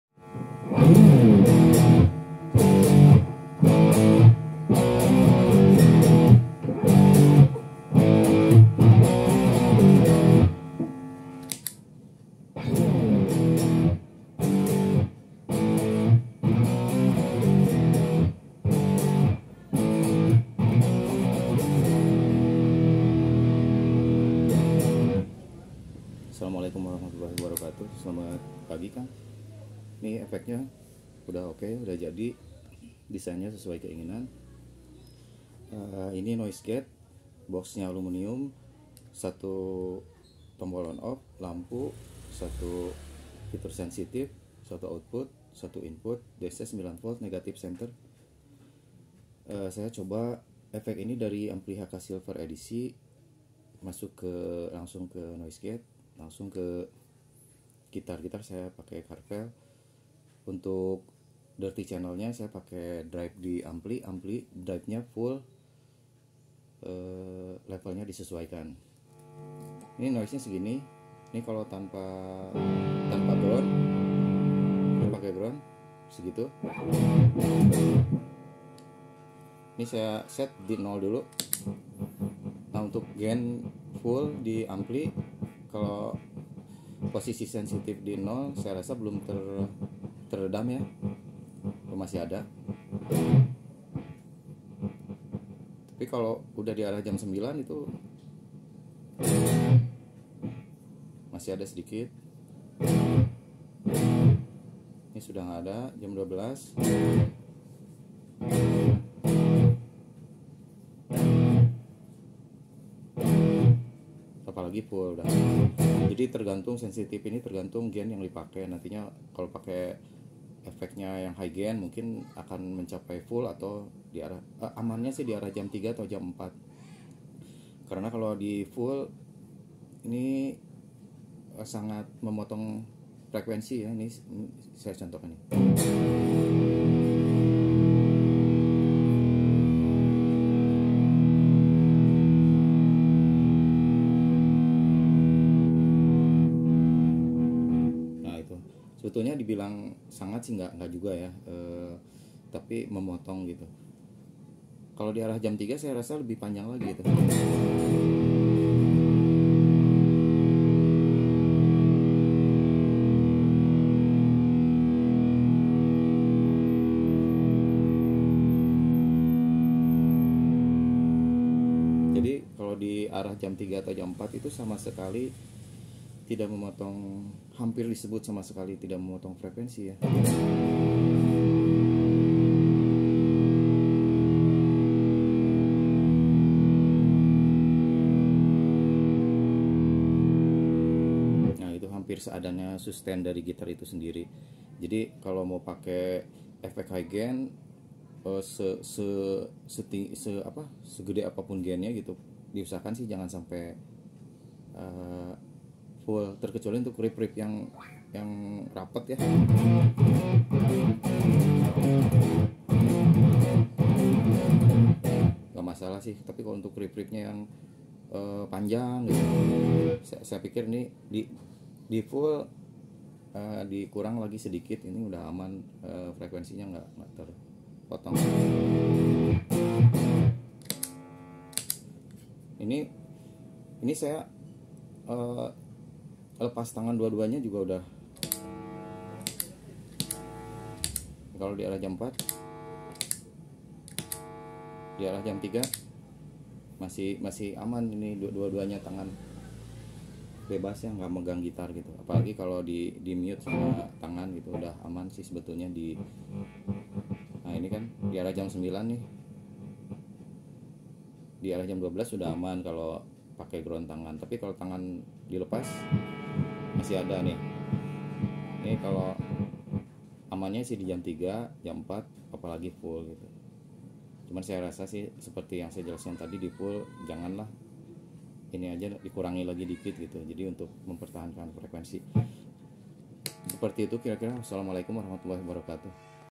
AHS noisegate siap meluncur ke palembang
Direkam menggunakan hape redmi 10, ampli h &k silver edition, gitar charvel

model-model efek cek instagram (heriebrton / ahs pedals)

untuk pemesanan silahkan via wa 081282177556